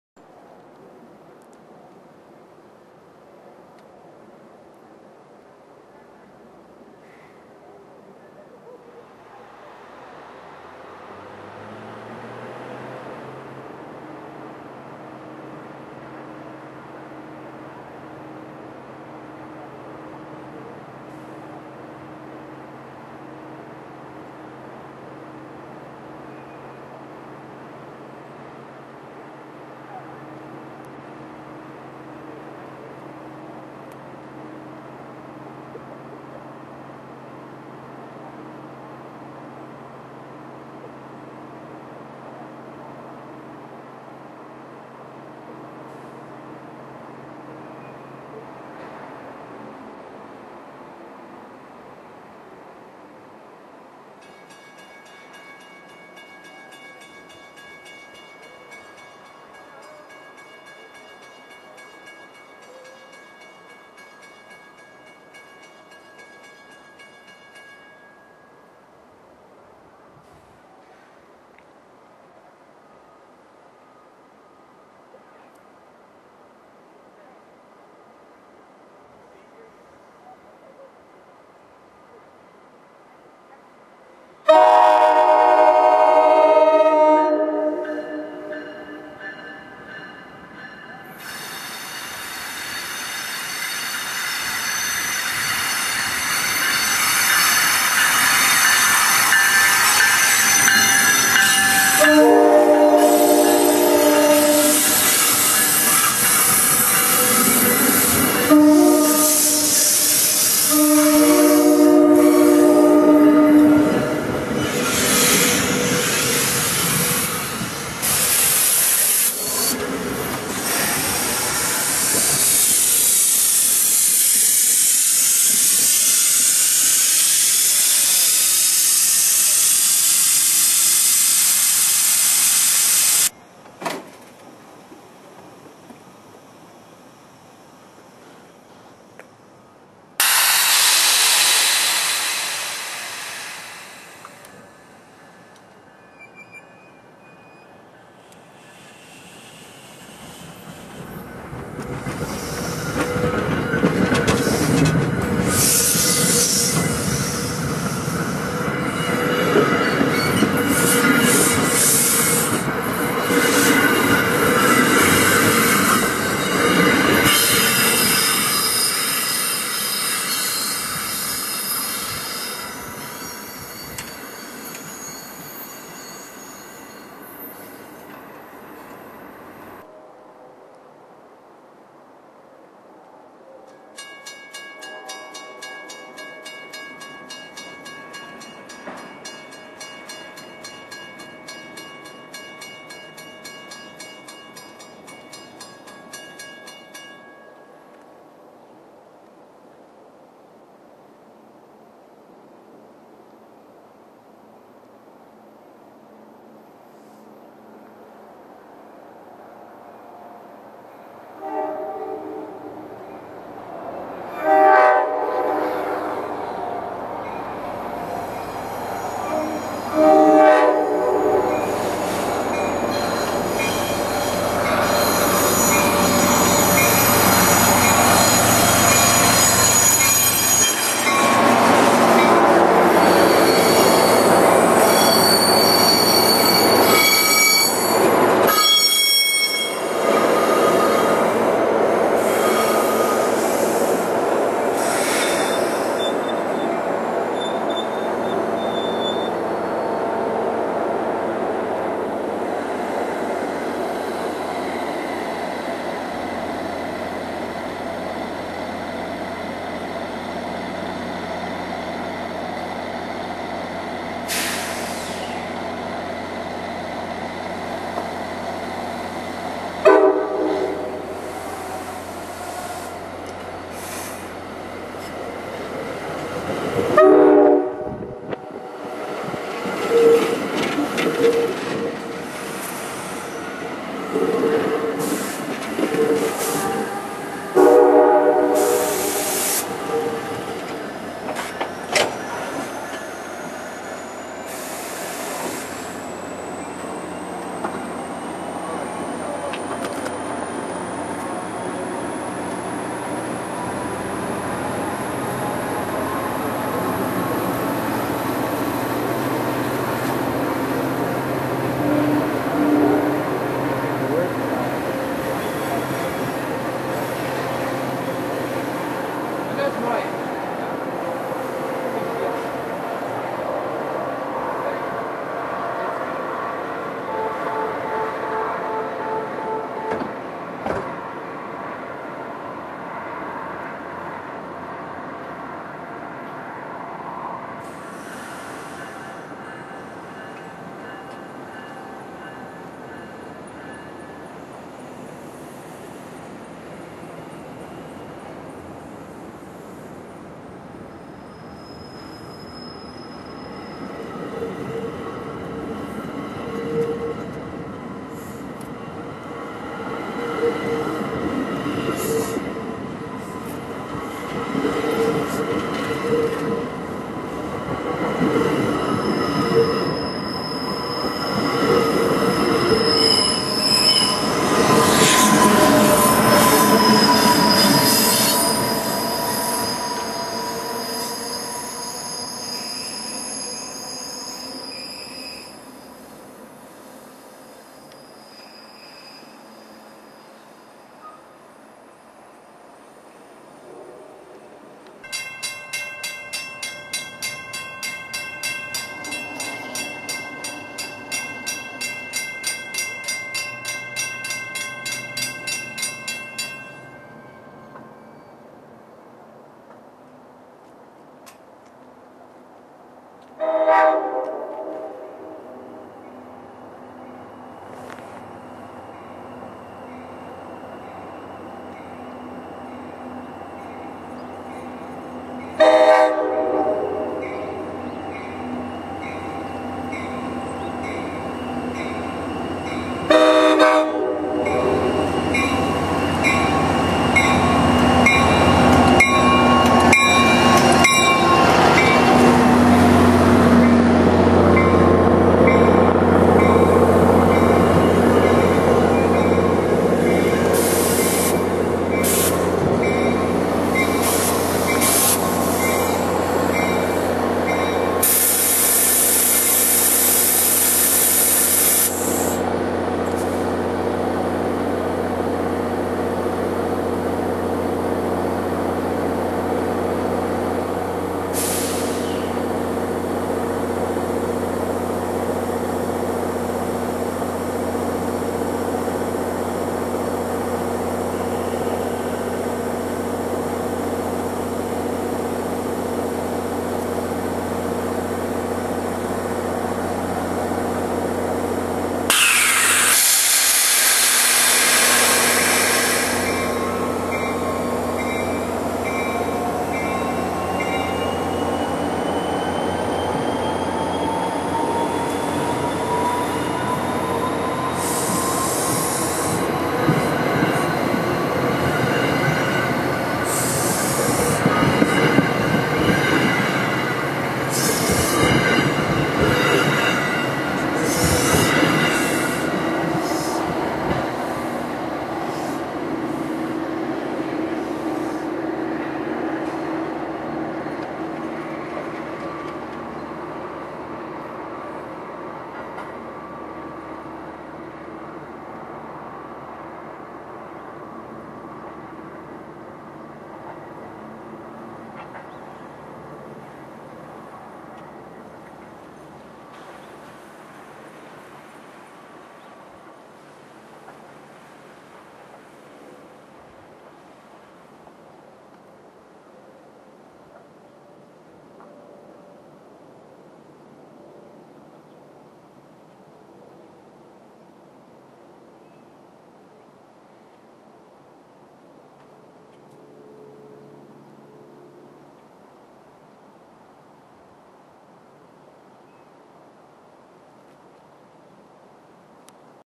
Denville Railfanning 1/31/11 Part 1
On the last day of January, I paid a visit to Denville for the first time in a month.

1:  Train 6650 pulls in on Track 1, running a few minutes late.  ALP-46A 4639 leads the way, and Cab Car 6063 is trailing.
2:  GP40FH-2 4138 leads Train 877 into the station with a friendly engineer and a burnt out ditchlight.  It meets Train 6654, led by Multilevel 7028.  ALP-46A 4640 powers 6654, and Cab Car 7008 is in front of it.  Cab Car 6034 is at the end of 877.
3:  Train 1001 arrives with GP40PH-2A 4149 on the point.  Trailing is Cab Car 6037.